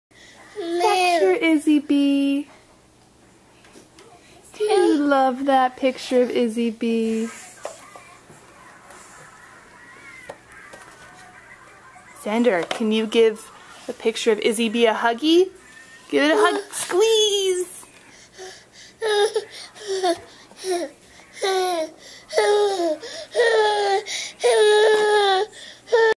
He loves his Izzy B!
Z loves to find the picture we have of Izzy B on our  bookshelf.  Then he gets mad because he misses her.  So precious!